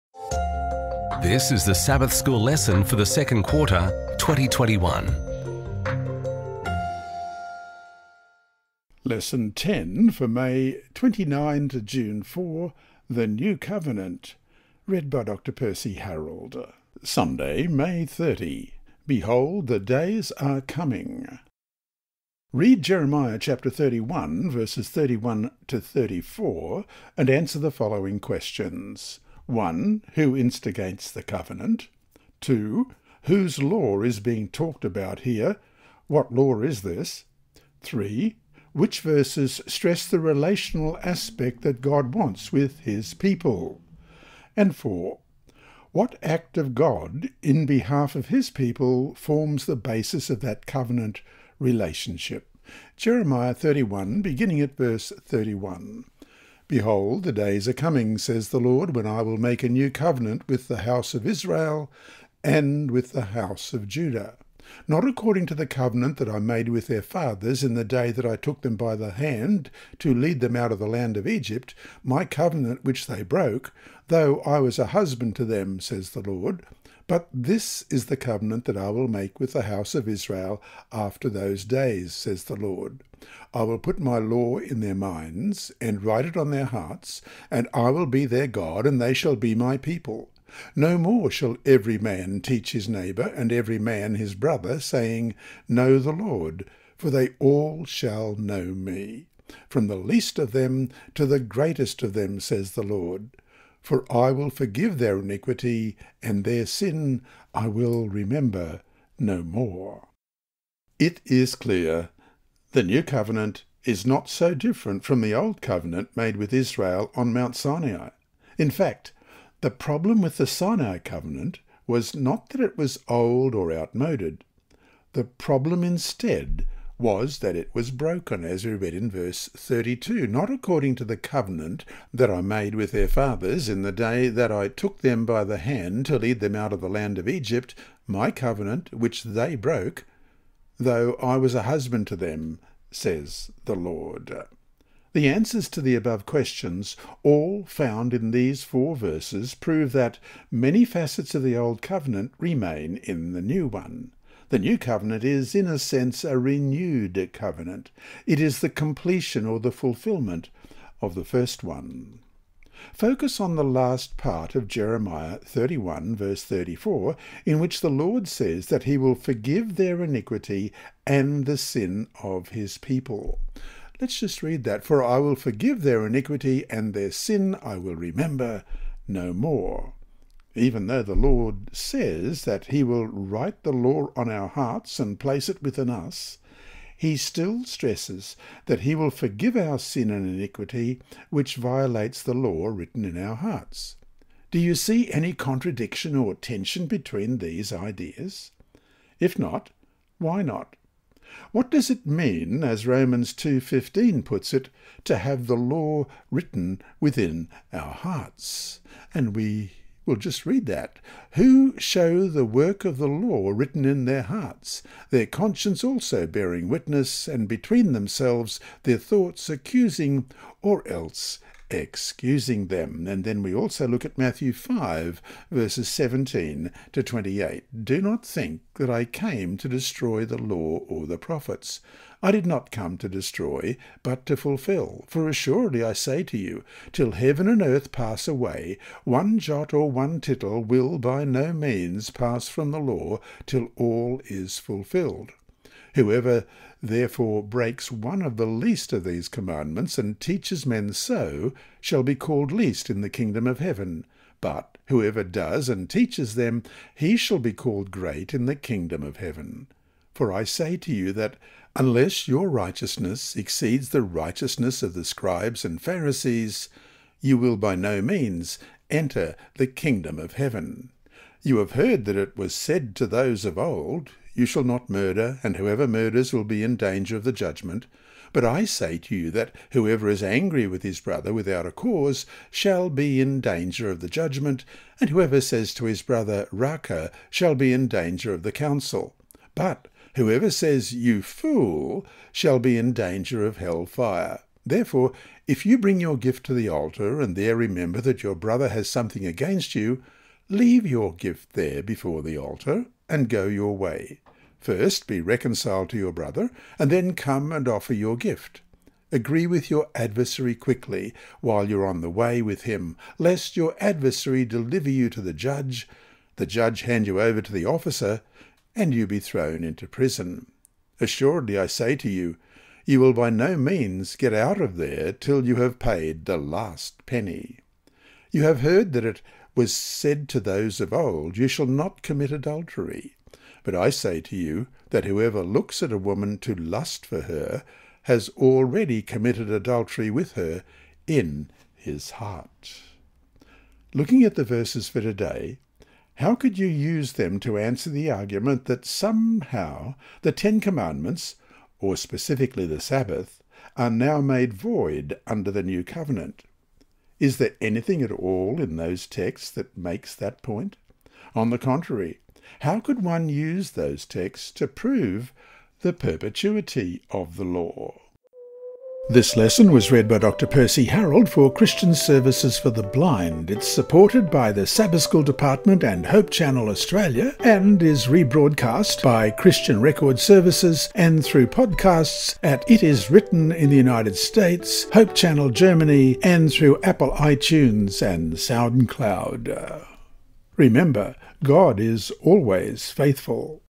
Sunday May 30, 2021: Behold, The Days Are Coming …
Lesson 10: The New Covenant
𝟐𝐧𝐝 𝐐𝐮𝐚𝐫𝐭𝐞𝐫 𝟐𝟎𝟐𝟏: 𝐓𝐡𝐞 𝐏𝐫𝐨𝐦𝐢𝐬𝐞 - 𝐆𝐨𝐝’𝐬 𝐄𝐯𝐞𝐫𝐥𝐚𝐬𝐭𝐢𝐧𝐠 𝐂𝐨𝐯𝐞𝐧𝐚𝐧𝐭

Today’s lesson was read by Dr. Percy Harrold from Queensland Australia.

The Advent-band Ministry 𝐃𝐎𝐄𝐒 𝐍𝐎𝐓 benefit financially from the posting of these videos.